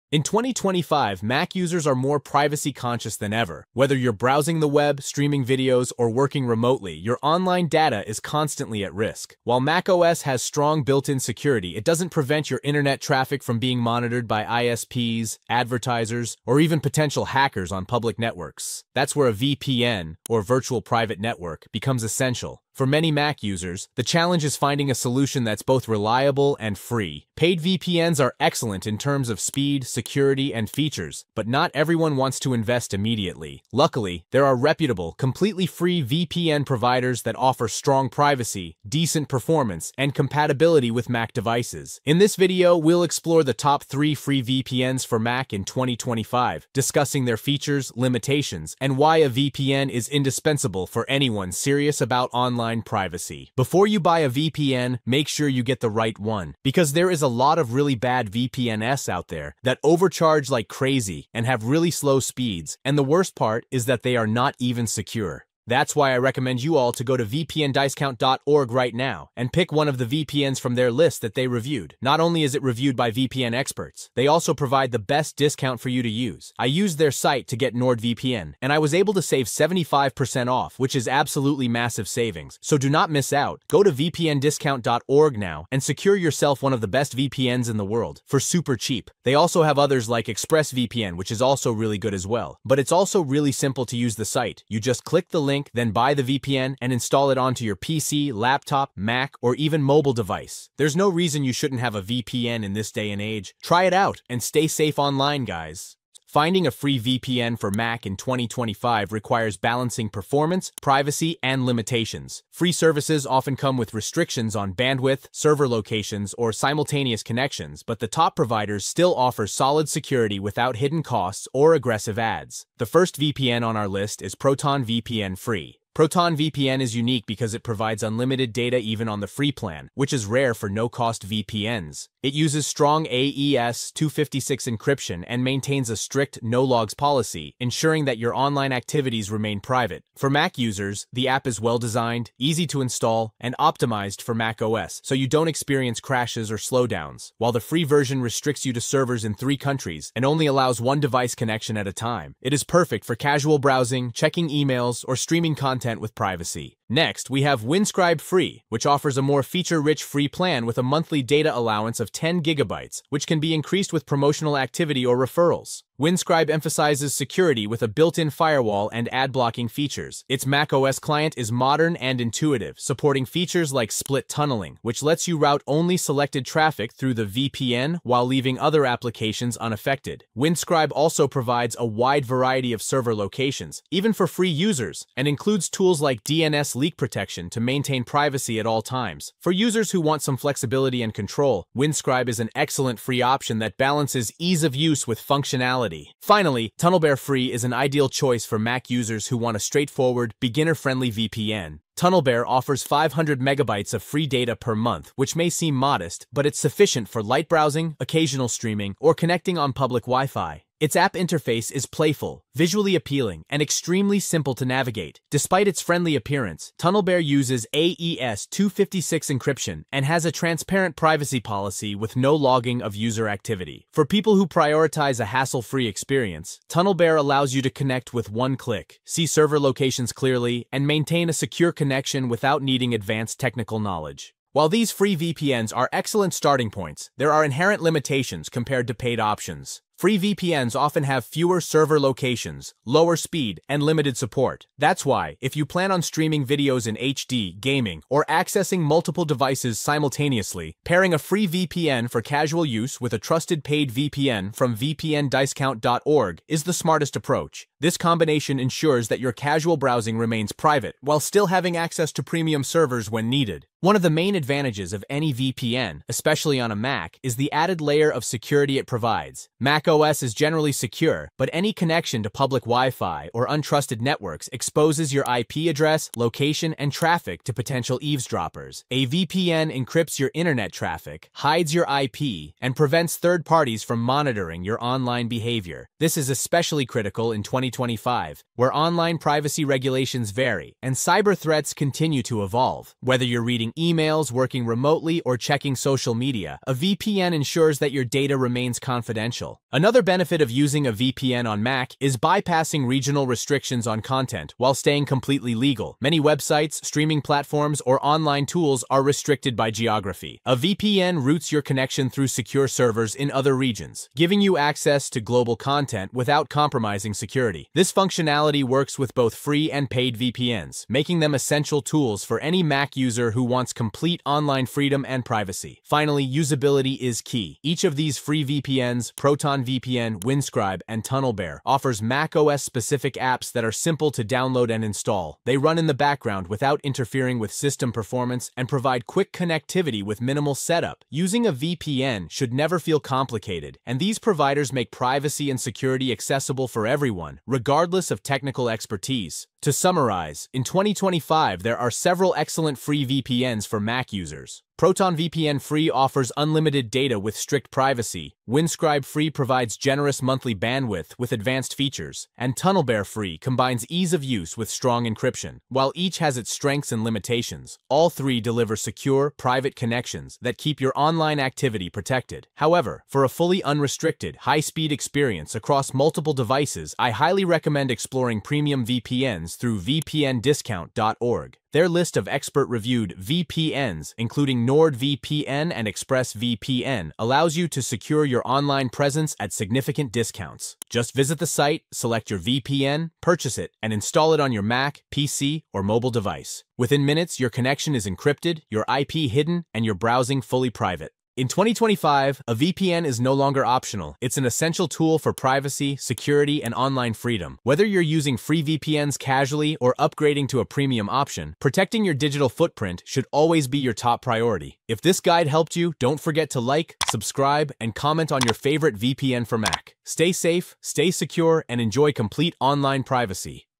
Free VPN For Mac - Top 3 Completely Free VPN Providers For MacOS
whats the best vpn for mac - best free vpn for mac , how to use vpn on mac,,  does nordvpn work on macbook

Discover the best free VPN for Mac in 2025 with this ultimate guide to the top 2 risk-free VPN providers for macOS—NordVPN and ExpressVPN! 🚀 If you're hunting for a completely free VPN Mac experience without upfront costs or hidden catches, leverage their generous 30-day money-back guarantees for unlimited, secure browsing on your MacBook or iMac—no credit card hassles during the trial period!
We break down why these premium powerhouses are the smartest "free" picks for macOS Sequoia and Sonoma users, delivering blazing speeds, ironclad privacy, and seamless native apps. Learn:
✅ NordVPN's 7-day mobile trial + 30-day refund: Access 6,000+ servers in 111 countries, unlimited bandwidth, and Threat Protection for malware-free streaming, torrenting, and gaming on Mac—retains 83% of your speeds!
✅ ExpressVPN's 30-day no-risk trial: LightSpeed servers for less than 13% speed loss, 3,000+ global hotspots, and Advanced Protection to crush trackers & ads—perfect for unblocking Netflix, Hulu, and geo-blocks effortlessly.
Ditch ISP snooping, public Wi-Fi risks, and slow connections. Both offer no-log policies audited 20+ times, split-tunneling, and easy one-click setup—ideal for secure free VPN Mac use in 2025, whether for work, travel, or privacy.
👉 Watch now to lock down your Mac in minutes—cancel anytime if it's not love at first connect!
💬 Drop your go-to free Mac VPN trial below!